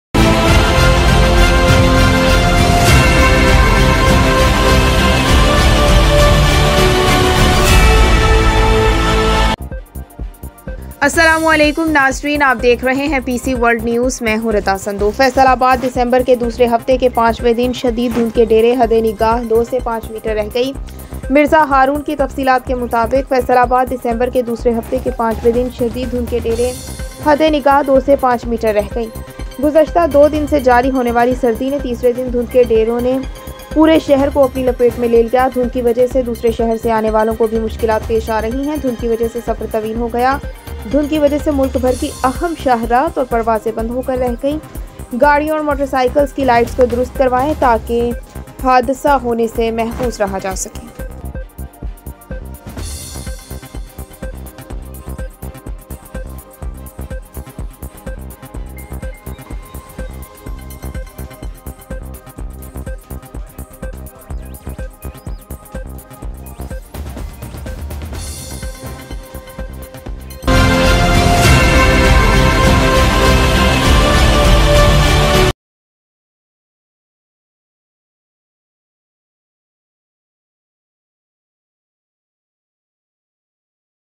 Faisalabad: On the fifth day of the second week of December, the fog limit is 2 to 5 meters
Pakistan Communicate World Network#FOG
A ISLAMIC NEWS CHANNEL